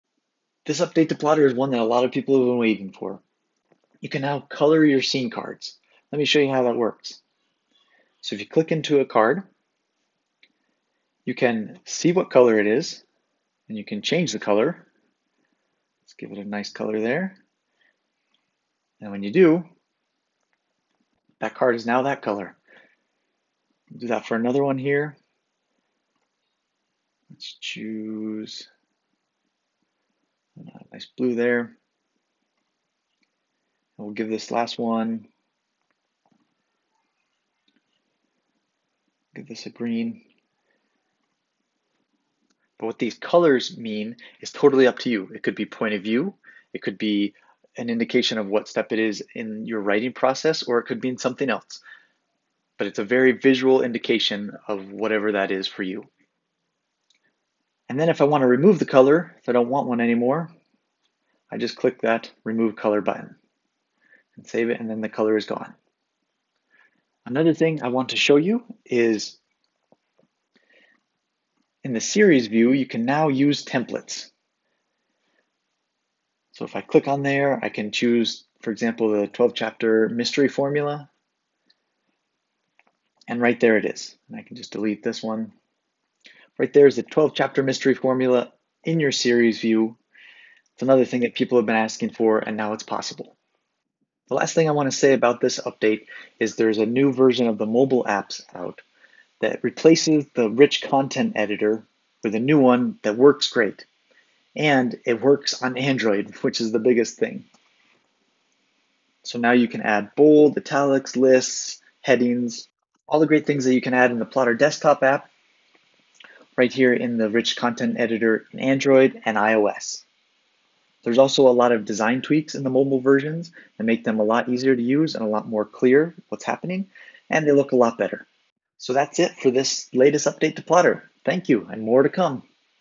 Color Your Scene Cards, Series View Templates, plus Mobile Improvements! | Release Notes 3.8.21
Our latest update (v3.8.21) brings you the highly requested ability to fill in the color of your scene cards PLUS use timeline templates within Series view! Not to mention, the iOS and Android apps have a fresh coat of paint and a brand new text editor. Do we need to say anything more? 🎨 🖌️